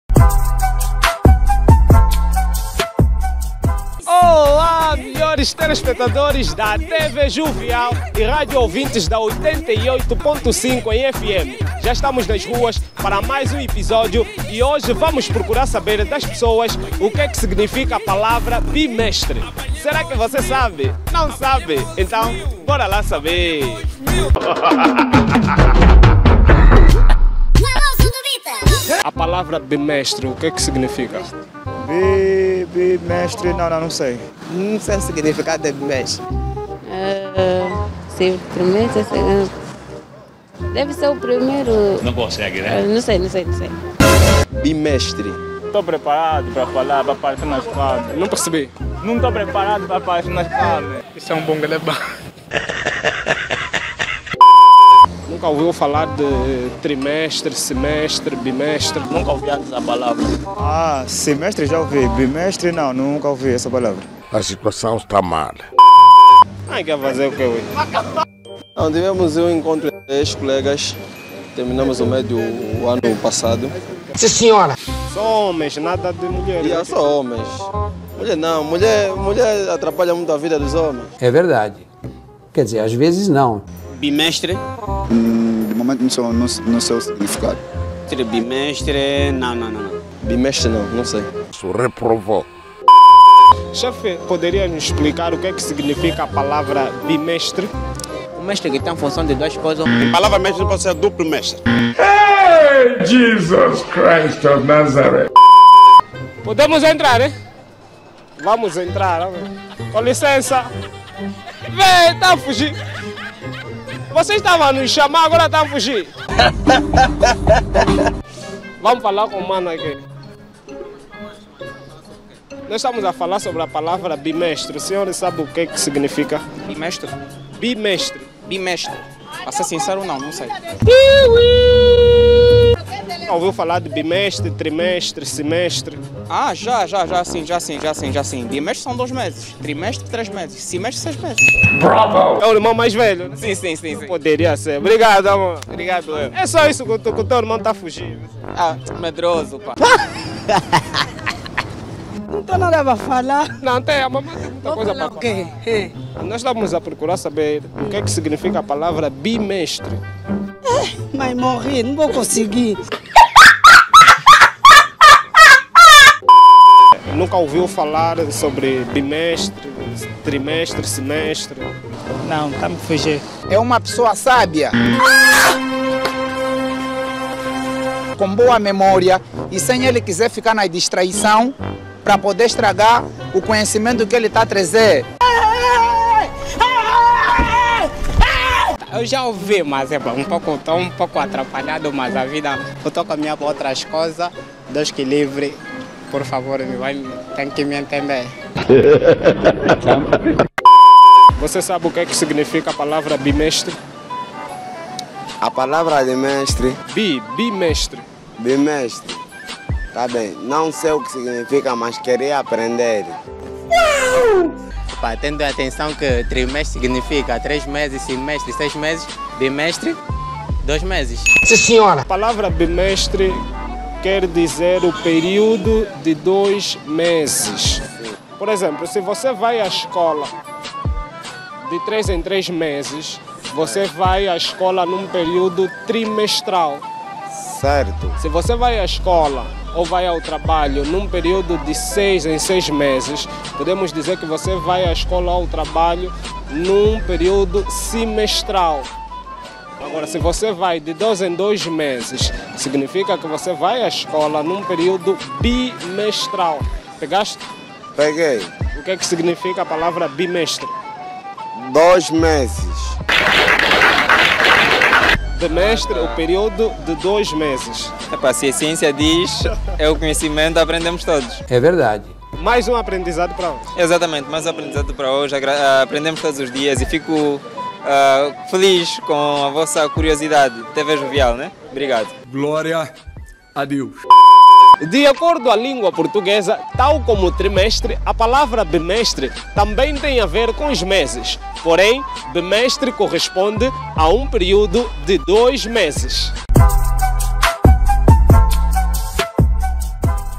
T2, Ep- 28 O quê que significa a palavra BIMESTRE?
TV JOVIAL - LINHA JOVEM

Melhores telespectadores da TV Jovial e ouvintes da Rádio Escola, o Microfone já está na Rua para mais um episódio.

Para este episódio, procuramos saber o significado da palavra Bimestre.
Será que as pessoas na rua sabem?

Acompanhe o programa até ao fim.

Microfone na Rua